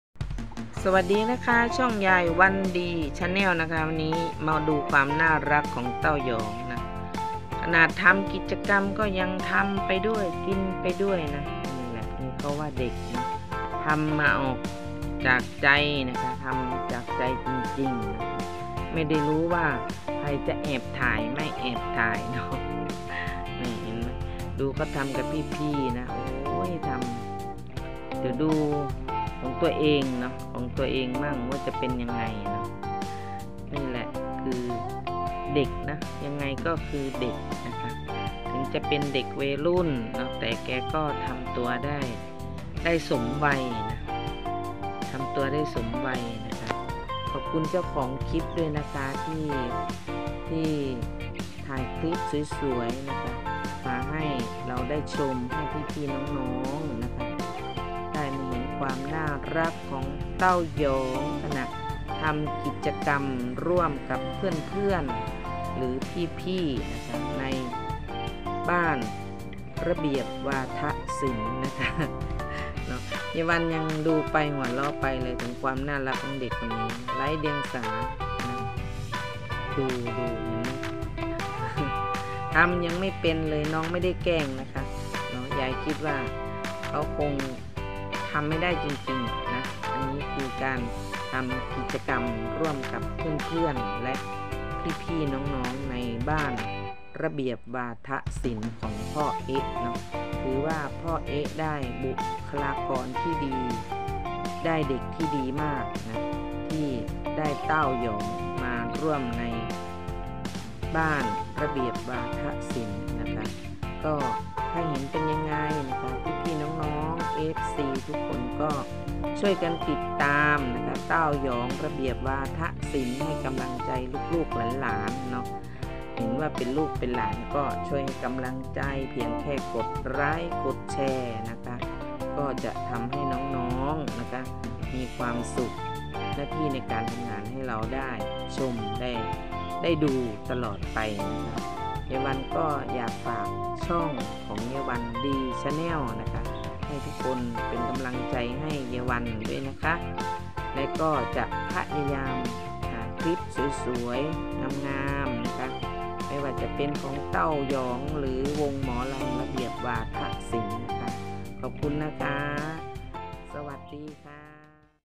ทำกิจกรรมร้องเพื่อนๆ
ที่ ..บ้านระเบียบวาทะศิลป์ ได้มีการให้เด็กๆร่วมกิจกรรมมากมาย มาดูความทะเล้น ความน่ารักของเด็กหนุ่มกำลังมาแรง..ต้าวหยอง